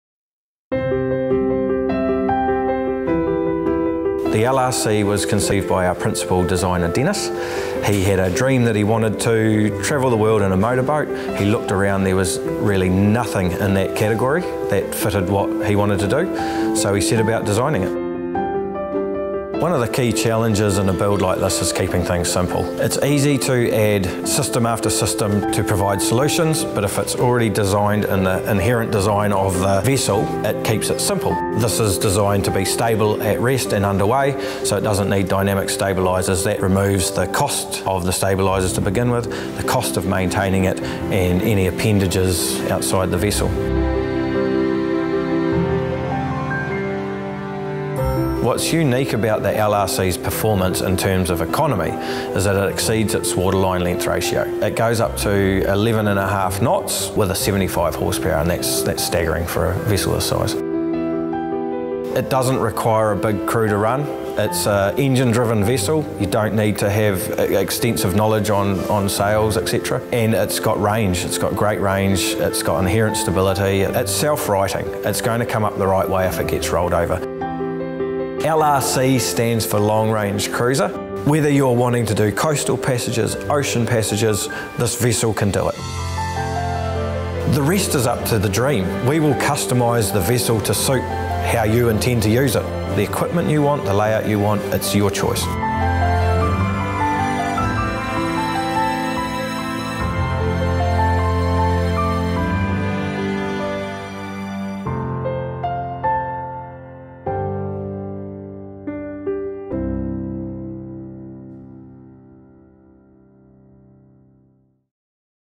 Dickey Boats Art Nautica LRC58
Dickey Boats are pleased to announce the launch of the new ArtNautica LRC 58. For more details view the teaser below or contact Jason Dickey on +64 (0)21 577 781.